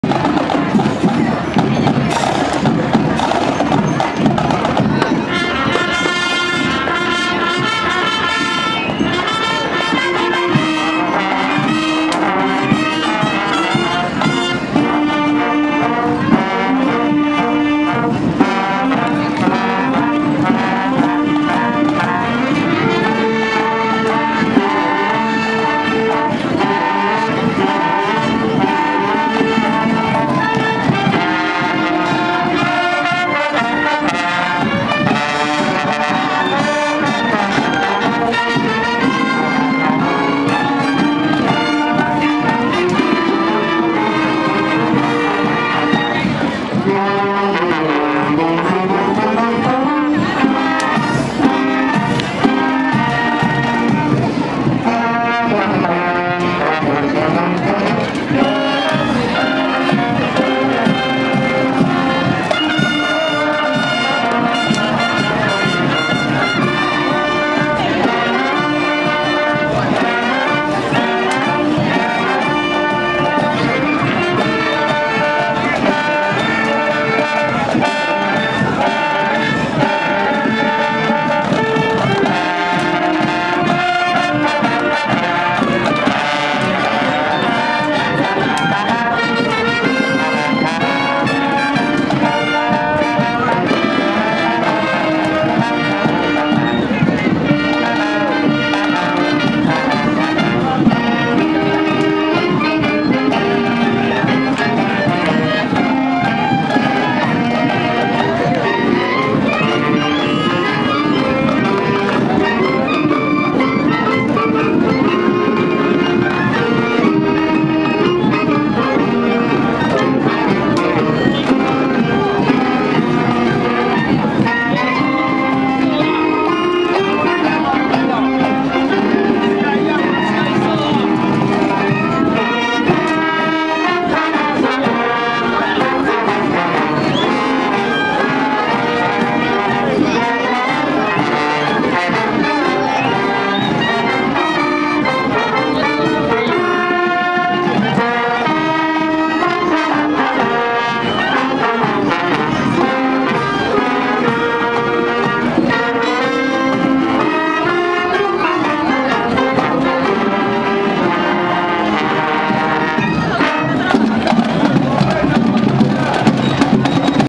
Banda de Música del Instituto Justo Arosemena 03.11.2014
Banda de Música del Instituto Justo Arosemena  en el desfile del 3 de Noviembre, Cinta Costera.